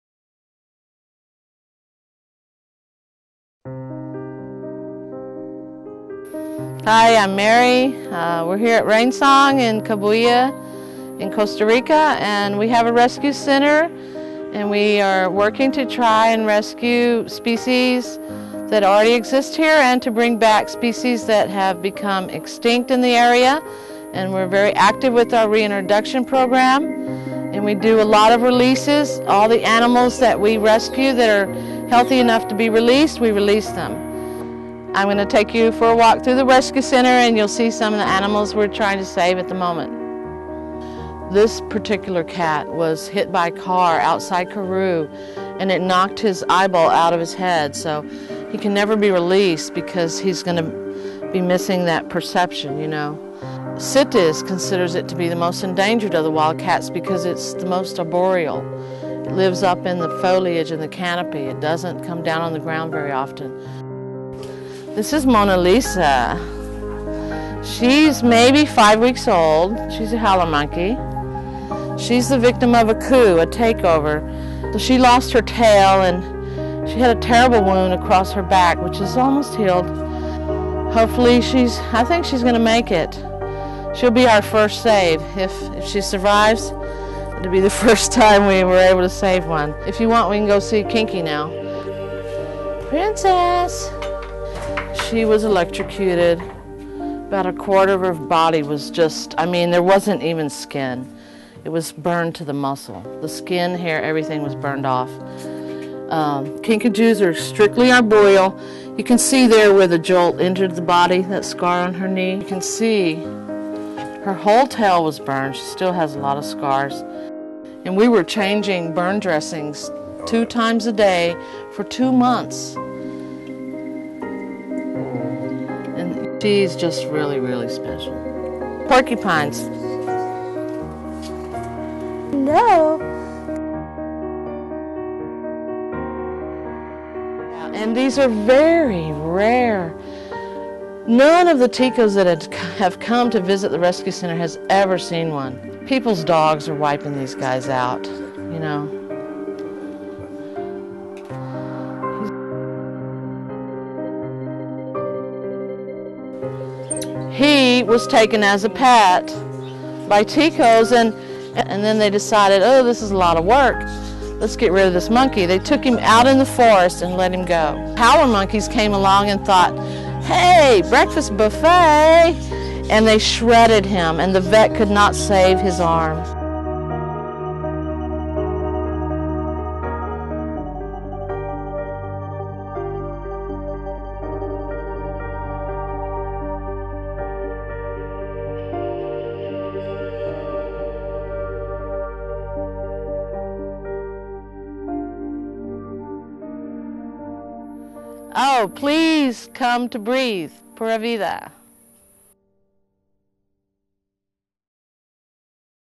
RAIN SONG - TEASER
Visiting mary from rain song and having a tour through their animal rescue center